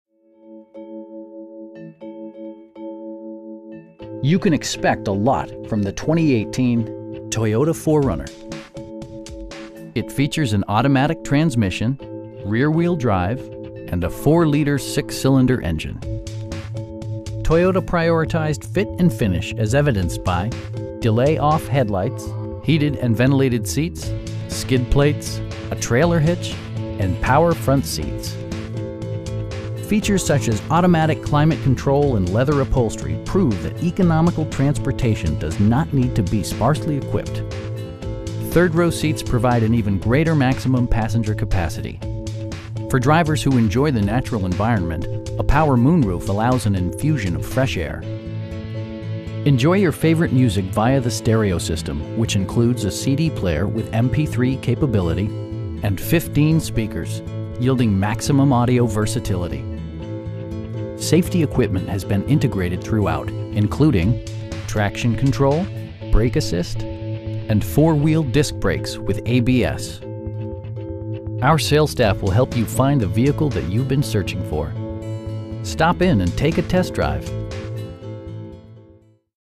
2018 Toyota 4Runner Limited in Waco, TX 76712
Jeff Hunter Toyota
1440 West Loop 340

You can expect a lot from the 2018 Toyota 4Runner. It features an automatic transmission, rear-wheel drive, and a 4 liter 6 cylinder engine. Toyota prioritized fit and finish as evidenced by: delay-off headlights, a rear window wiper, heated and ventilated seats, telescoping steering wheel, skid plates, a trailer hitch, and power front seats. Features such as automatic climate control and leather upholstery prove that economical transportation does not need to be sparsely equipped. Third row seats provide an even greater maximum passenger capacity. For drivers who enjoy the natural environment, a power moon roof allows an infusion of fresh air. Safety equipment has been integrated throughout, including: dual front impact airbags with occupant sensing airbag, front side impact airbags, traction control, brake assist, anti-whiplash front head restraints, a security system, an emergency communication system, and 4 wheel disc brakes with ABS. Electronic stability control ensures solid grip atop the road surface, no matter how challenging the driving conditions. Our sales staff will help you find the vehicle that you've been searching for. We'd be happy to answer any questions that you may have. Stop in and take a test drive!